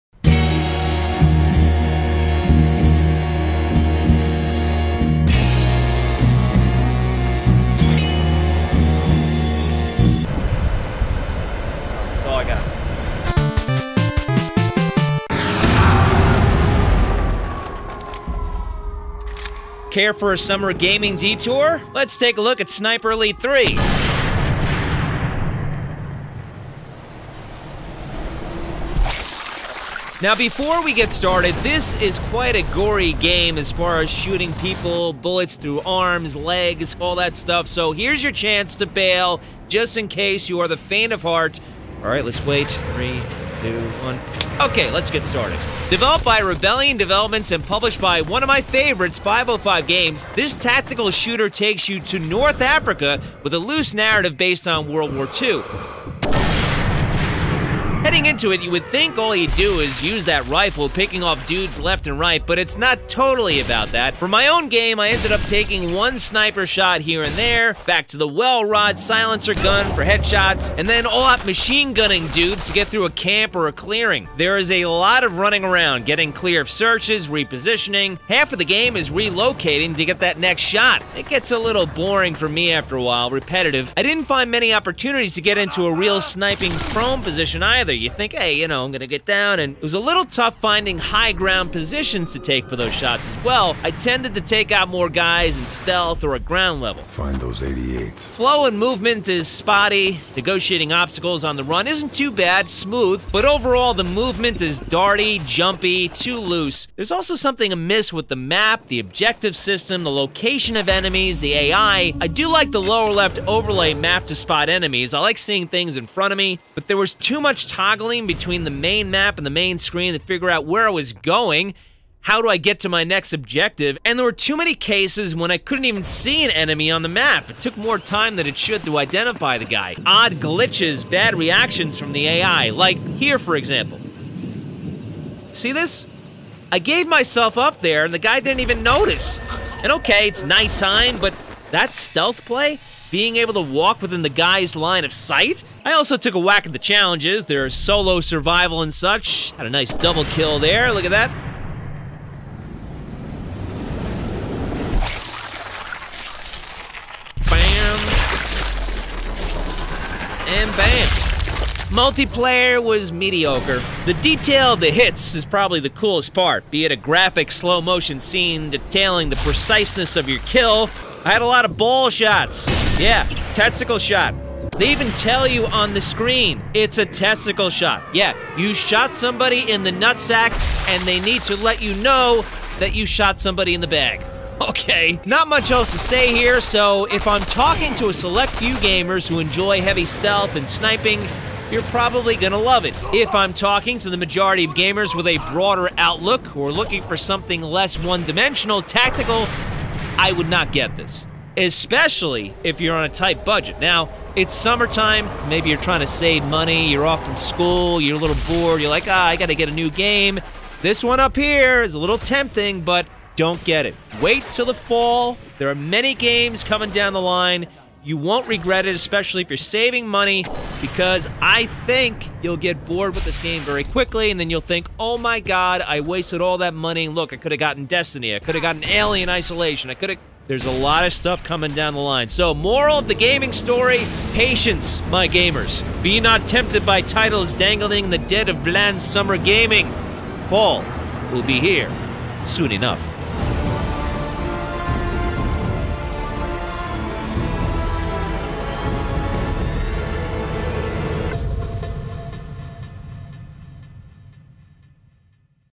Sniper Elite 3 Review
A look at Sniper Elite III from Rebellion/505 Games